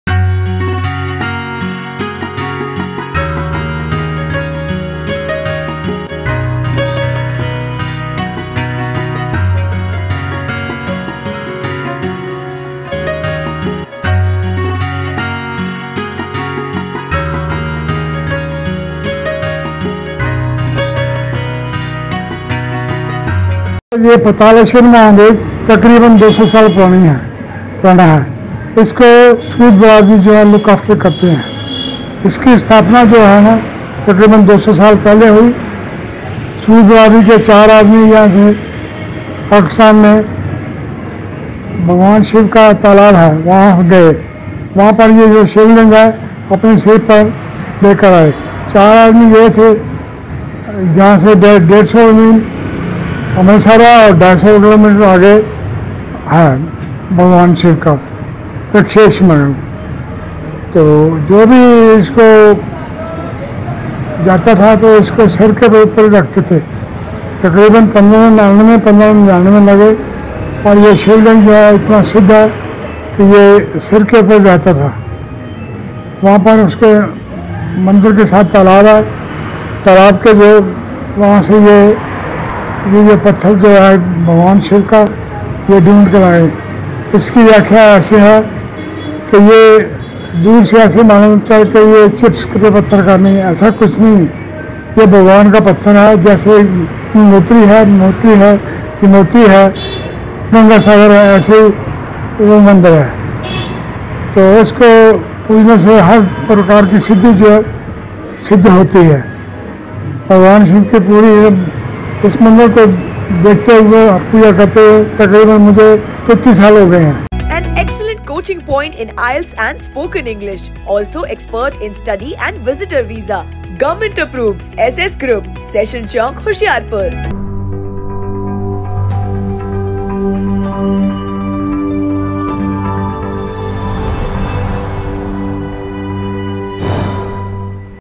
श्री पतालेश्वर मंदिर में बड़ी संख्या में श्रद्धालु हुए नतमस्तक
महाशिवरात्रि पर्व के उपलक्ष्य में श्री पतालेश्वर महादेव मंदिर ऊना रोड होशियारपुर में बड़ी संख्या में श्रद्धालु हुए नतमस्तक। मंदिर प्रबंधकों ने मंदिर के इतिहास के बारे में लोगों को करवाया अवगत। गुरजीत सोनू की रिपोर्ट। 8 मार्च 2024